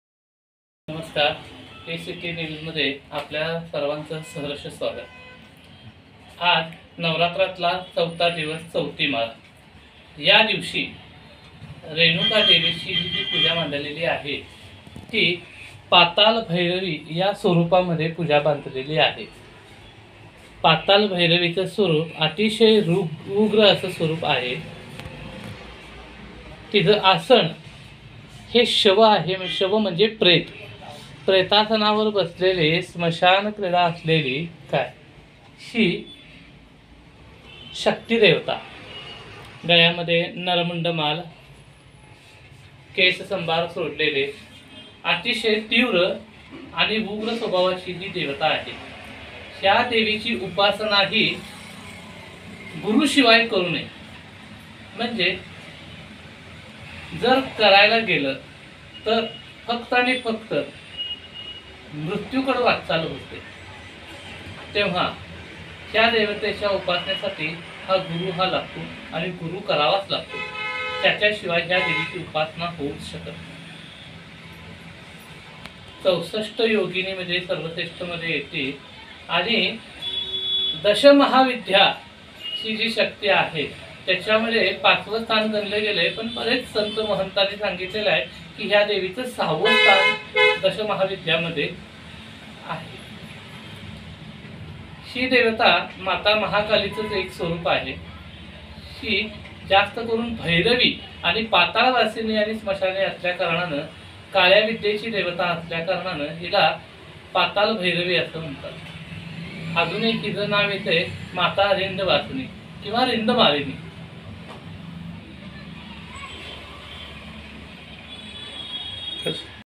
कोल्हापूर | चौथी माळ | शारदीय नवरात्रौत्सव २०२२
कोल्हापूर २९ सिटी न्यूज नेटवर्क
शारदीय नवरात्रौत्सव आजच्या दिवशी श्री रेणुका मातेची पाताल भैरवी रुपात पूजा केली आहे .

विशेष सहकार्य आदित्य बूजरे